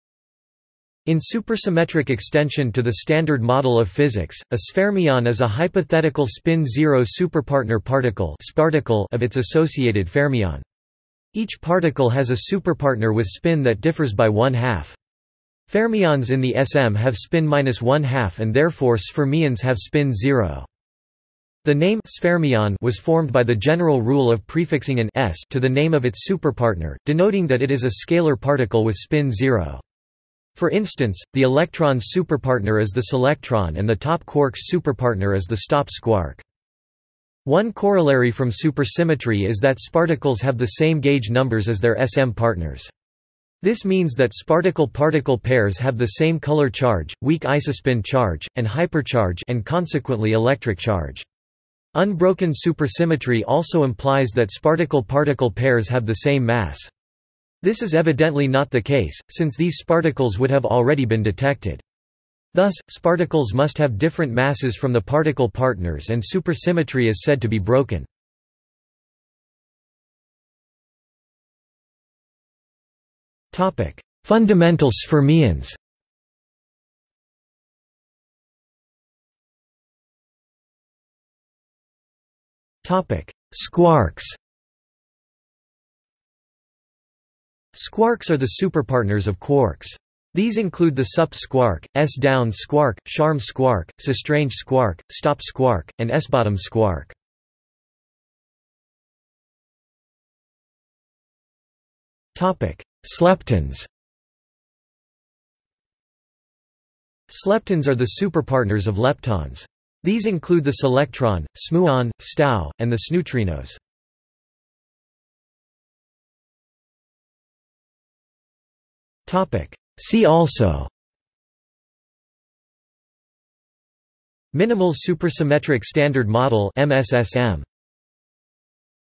Sfermion | Wikipedia audio article
This is an audio version of the Wikipedia Article:
Sfermion

00:01:09 1 Fundamental sfermions
00:01:19 1.1 Squarks
00:01:39 1.2 Sleptons
00:01:55 2 See also

- Socrates

SUMMARY
In supersymmetric extension to the Standard Model of physics, a sfermion is a hypothetical  spin-0 superpartner particle (sparticle) of its associated fermion.  Each particle has a superpartner with spin that differs by 1/2. Fermions in the SM have spin-1/2 and therefore sfermions have spin 0.
The name 'sfermion' was formed by the general rule of prefixing an 's' to the name of its superpartner, denoting that it is a scalar particle with spin 0. For instance, the electron's superpartner is the selectron and the top quark's superpartner is the stop squark.
One corollary from supersymmetry is that sparticles have the same gauge numbers as their SM partners. This means that sparticle–particle pairs have the same color charge, weak isospin charge, and hypercharge (and consequently electric charge). Unbroken supersymmetry also implies that sparticle–particle pairs have the same mass. This is evidently not the case, since these sparticles would have already been detected. Thus, sparticles must have different masses from the particle partners and supersymmetry is said to be broken.